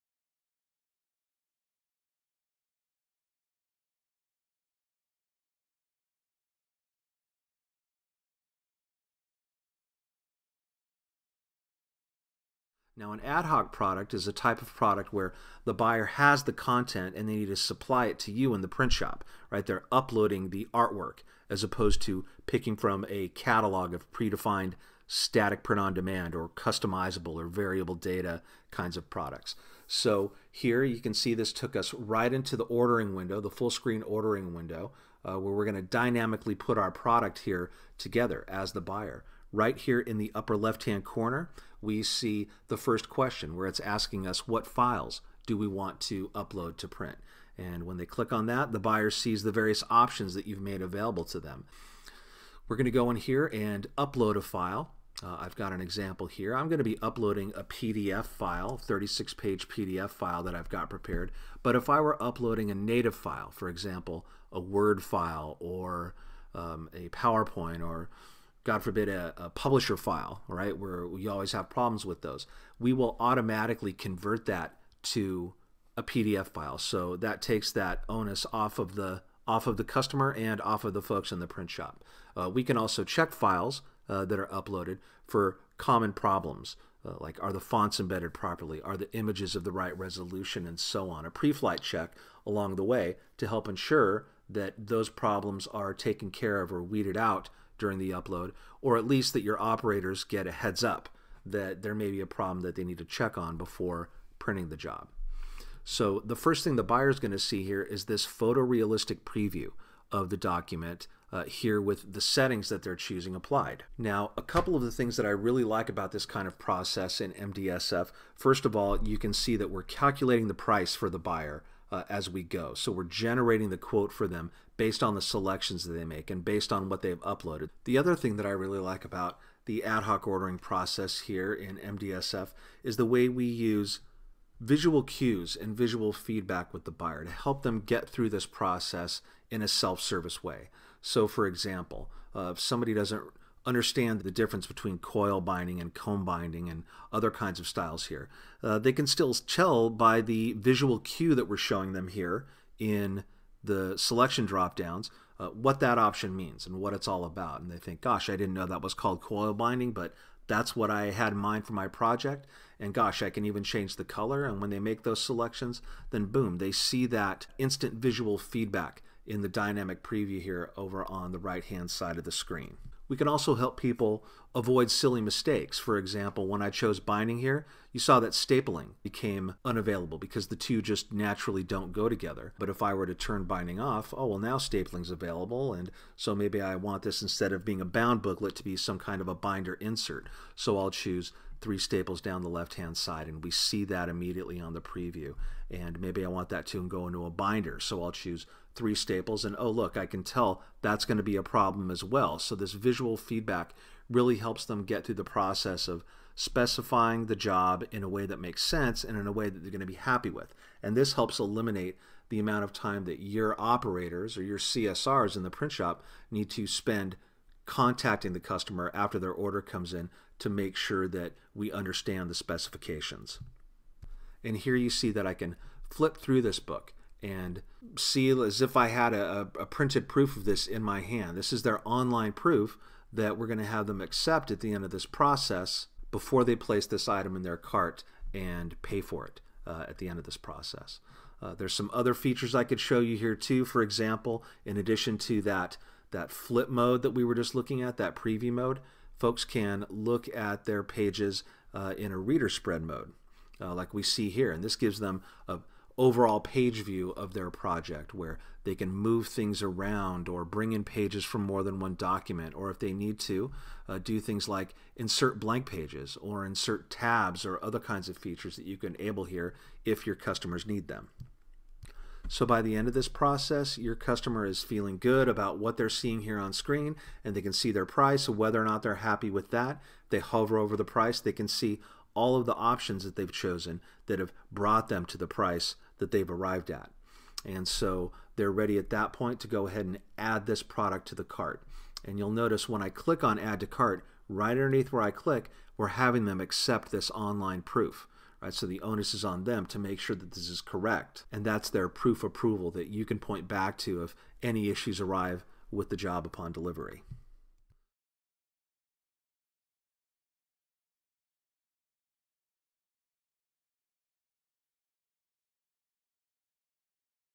Understanding Adhoc Products in EFI MarketDirect StoreFront
EFI MarketDirect StoreFront supports Adhoc product types, which consist of customer supplied artwork combined with powerful document building tools. You can specify which options each product has, and build out comprehensive configuration options for each. As customers select different options, pricing is automatically adjusted making this the leading online "on the fly" dynamic print pricing engine.